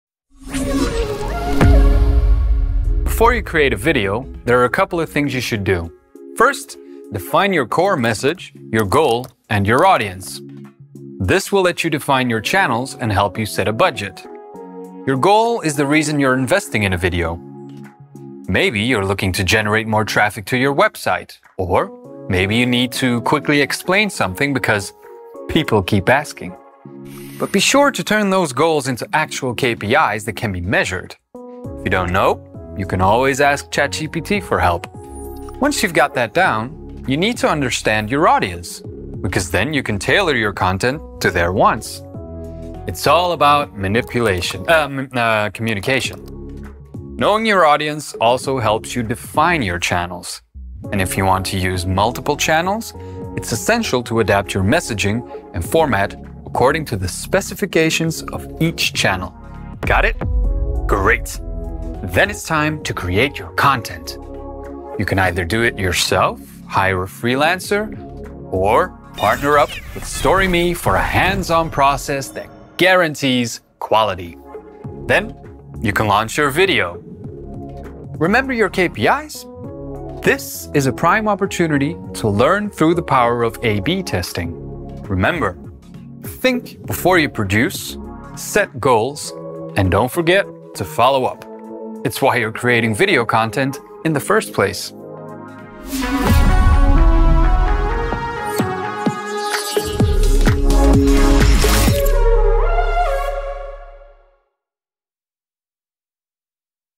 How To Get Started With Video | FAQ #14
Welcome to our FAQ series, where our host Thomas answers your most burning questions about video strategy, video marketing and StoryMe.

In this episode of FAQ, Thomas unravels the secret behind getting started with your video strategy.